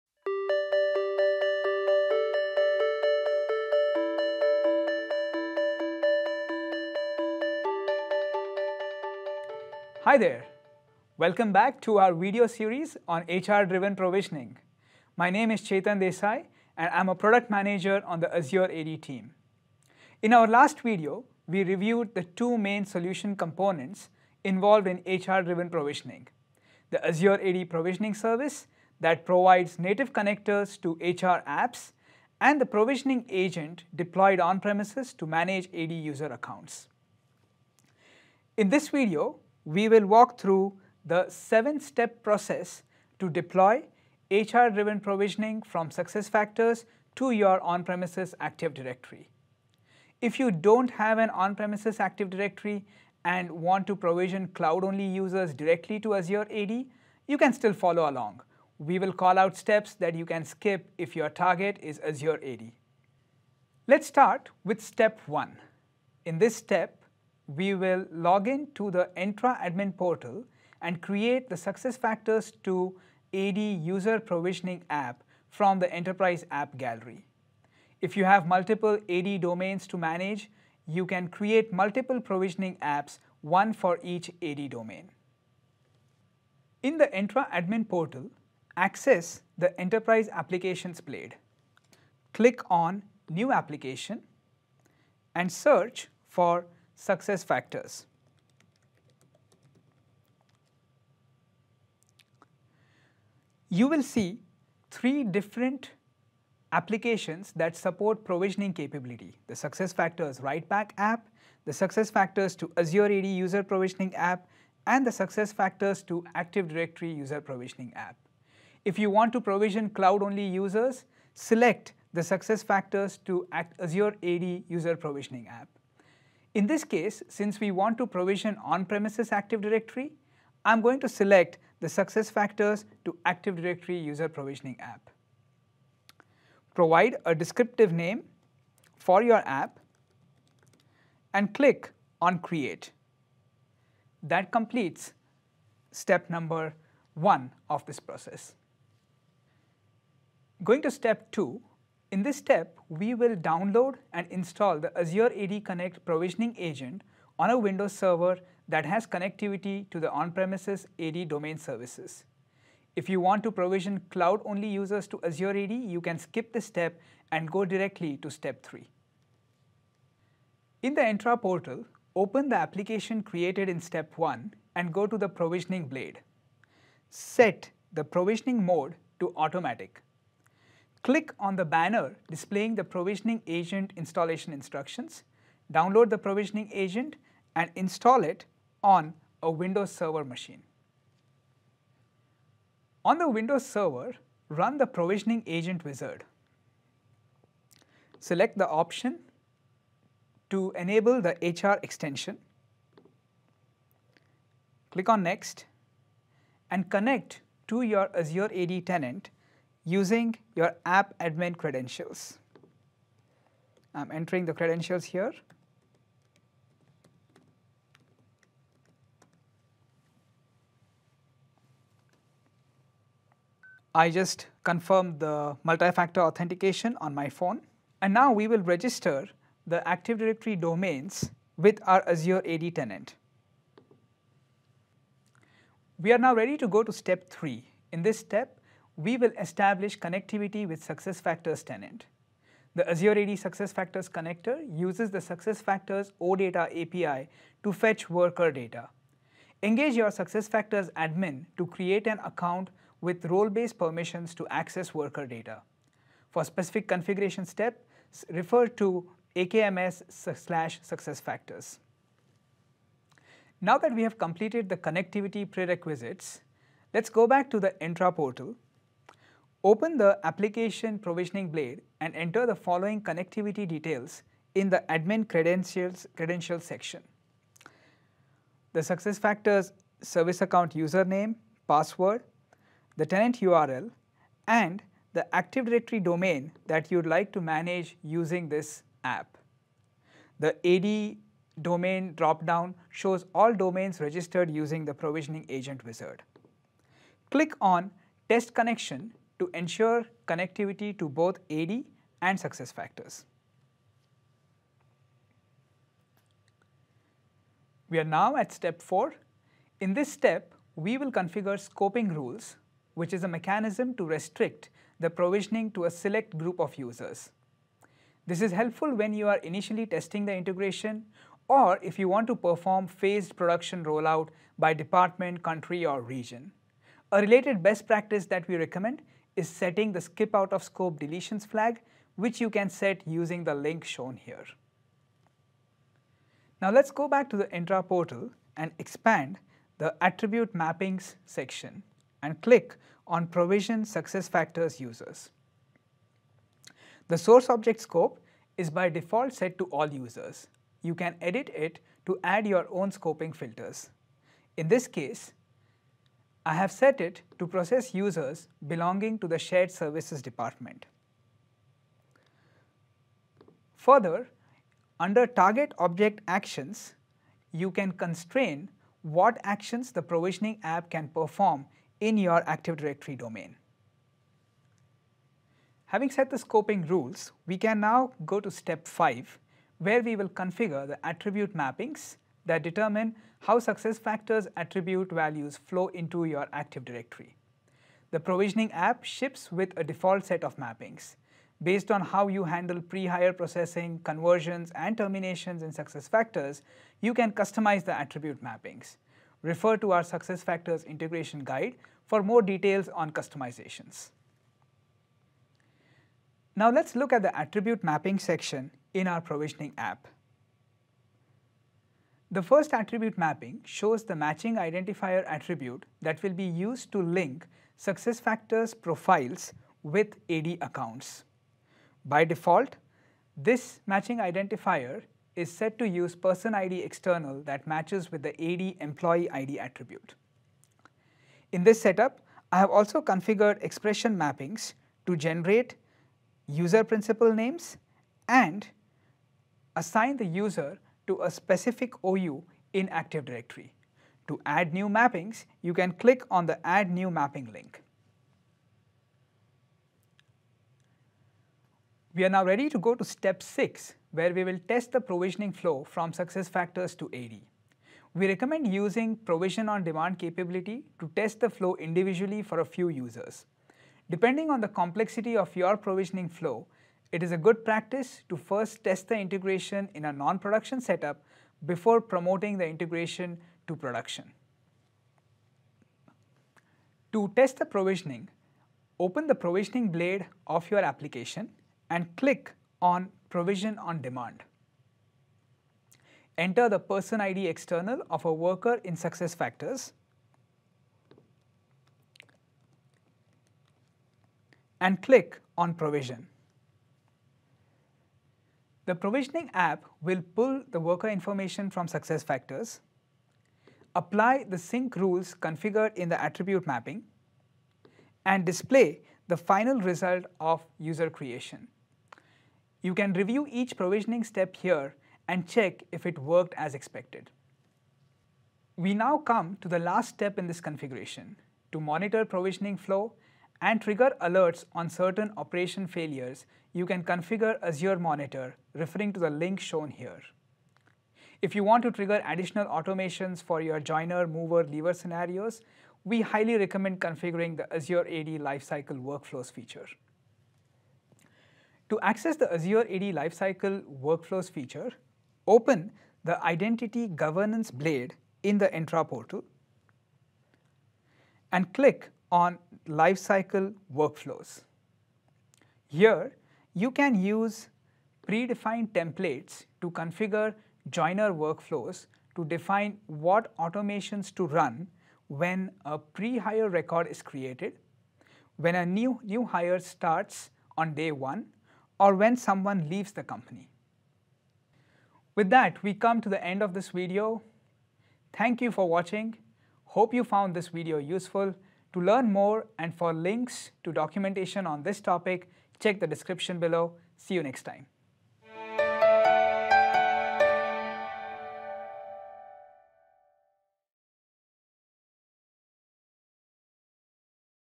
HR-driven Provisioning with Microsoft Entra ID
As enterprises adapt to changing times, IT and business teams are increasingly reliant on accurate and consistent identity data from HR to enhance employee experiences in areas such as new hire onboarding, work location transitions and offboarding. Entra ID identity provisioning service offers native integrations with Cloud HCM providers such as Workday and SuccessFactors. In this 3-part series, we will go through the steps involved in enabling HR-driven user provisioning with Entra ID and demonstrate how you can use capabilities such as enhanced attribute support, flexible mapping rules, and writeback support to automate the flow of identity data between HR and IT systems.